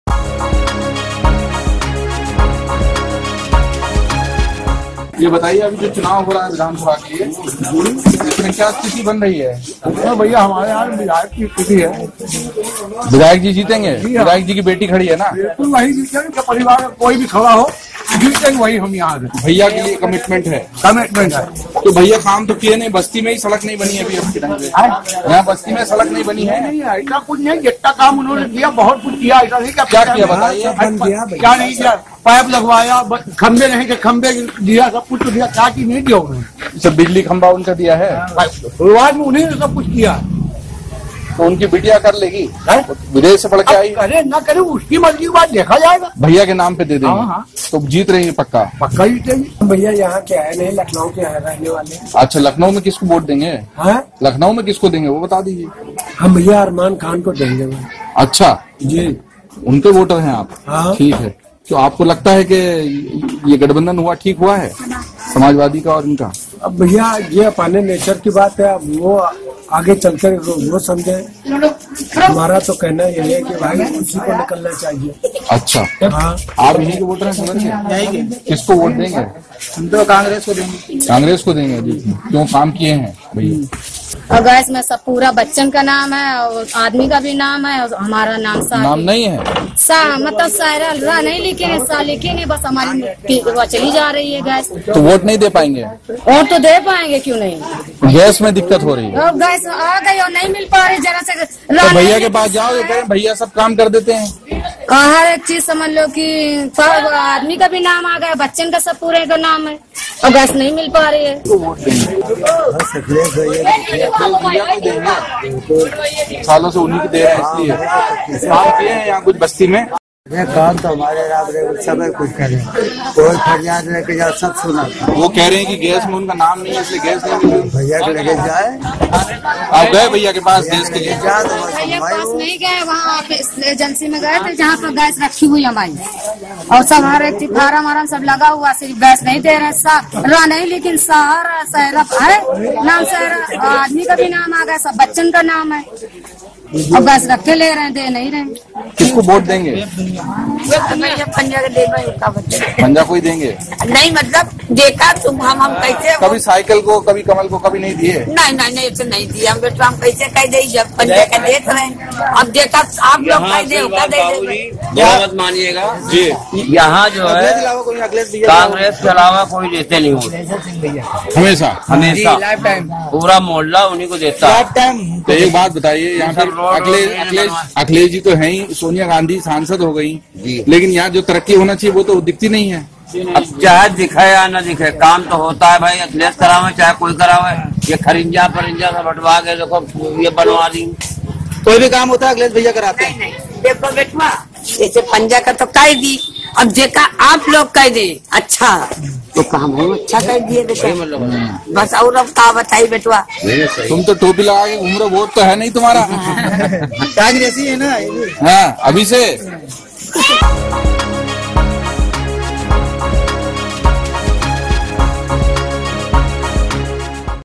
यूपी चुनाव : यहां तो भैया का बोलबाला है... वीडियो-रिपोर्ट : जयदीप कर्णिक (संपादक, वेबदुनिया)
* रायबरेली की सदर सीट पर बाहुबली अखिलेश सिंह का बोलबाला
* यहां से अखिलेश की बेटी अदिति सिंह कांग्रेस के टिकट पर चुनाव लड़ रही हैं
* रायबरेली का मुस्लिम इलाका बैराना बस्ती में अदिति को एकतरफा समर्थन
* अखिलेश सिंह यहां से 5 बार विधायक रह चुके हैं